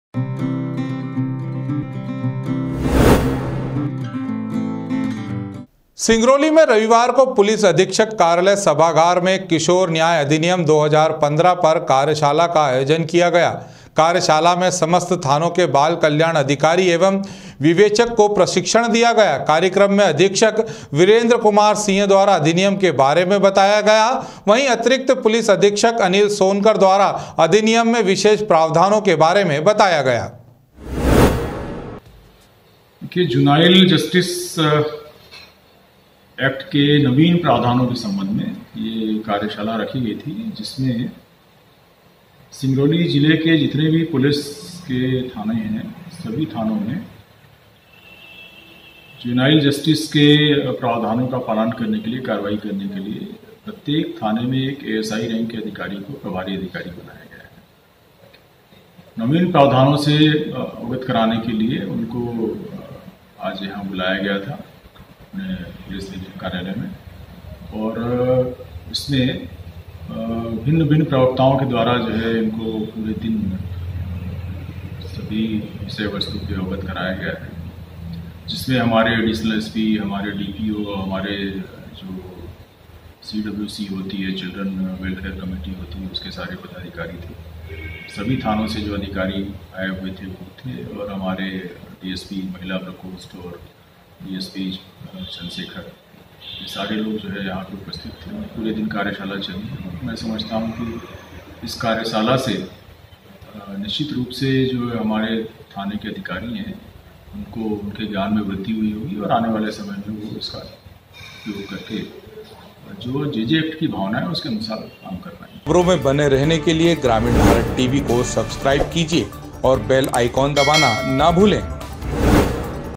किशोर न्याय अधिनियम 2015 पर कार्यशाला का आयोजन Organized Workshop on Juvenile Justice Act 2015
किशोर न्याय अधिनियम 2015 पर कार्यशाला का आयोजन
बाल कल्याण अधिकारियों एवं विवेचक को दिया प्रशिक्षण
एसपी वीरेंद्र कुमार सिंह, एएसपी अनिल सोनकर रहे उपस्थित